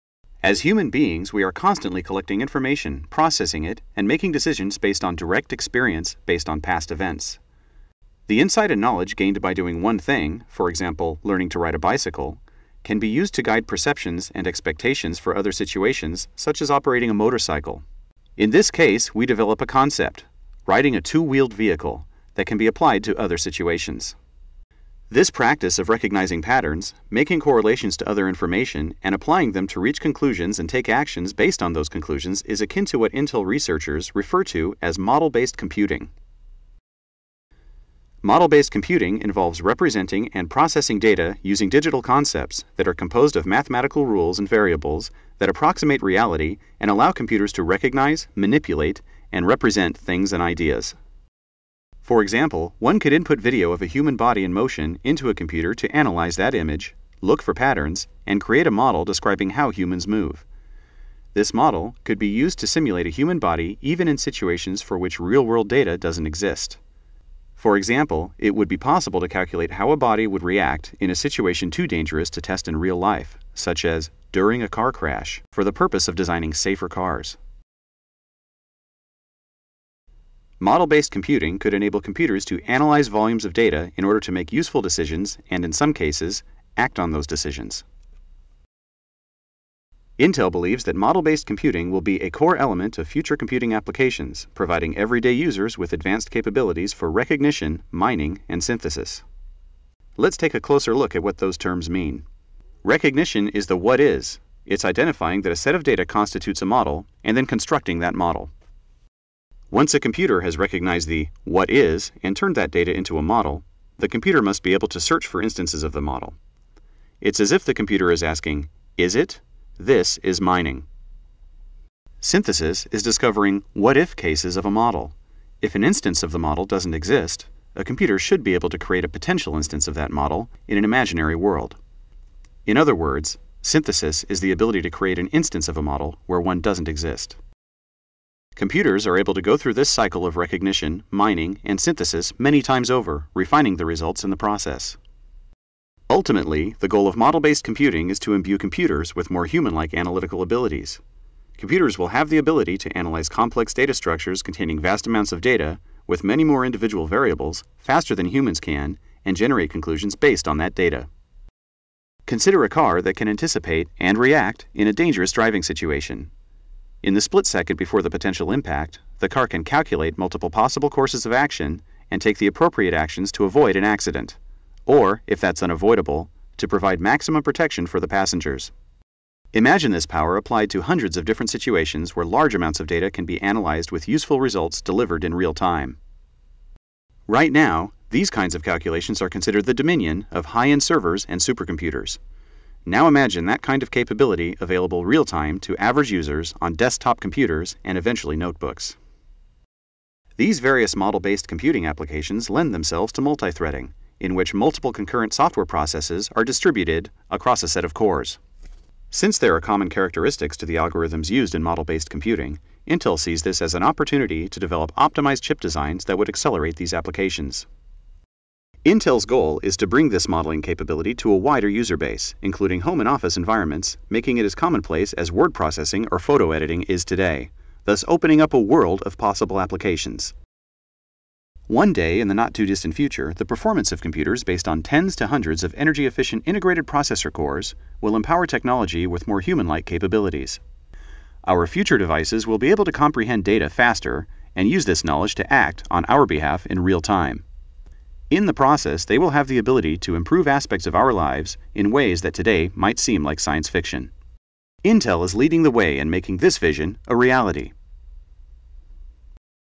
Model-based Computing
Description of an emerging class of applications Intel researchers call "model-based" computing.